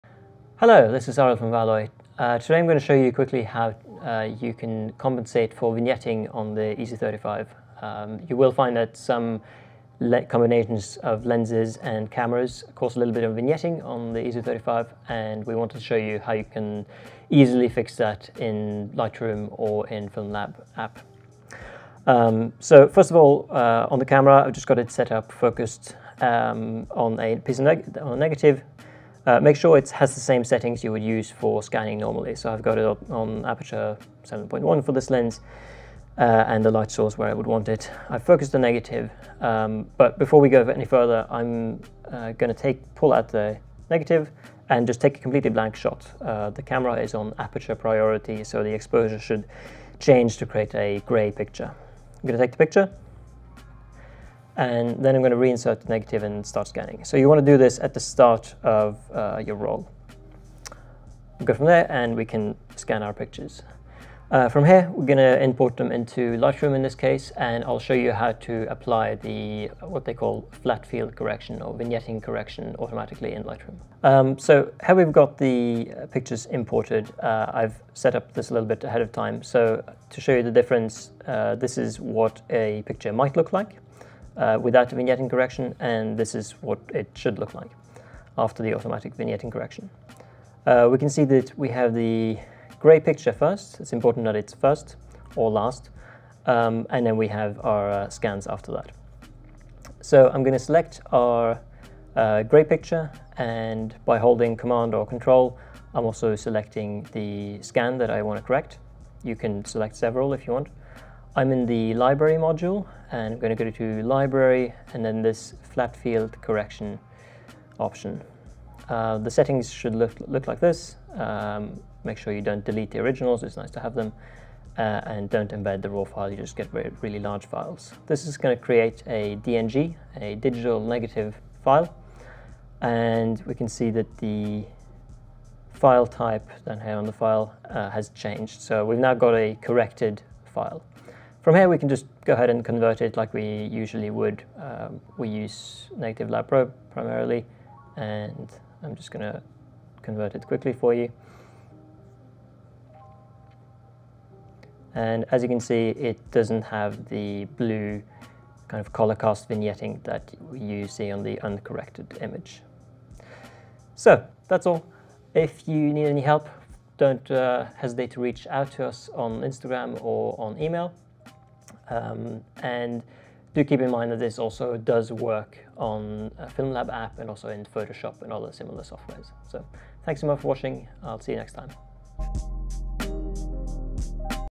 VALOI easy35 - Compensate for vignetting
This video shows you how to compensate for vignetting where the corners are bright and/or have a colour cast when scanning with the easy35. The instructions are using Lightroom, but other editing software has similar functionality, such as in Grain2Pixel for Photoshop and FilmLab App.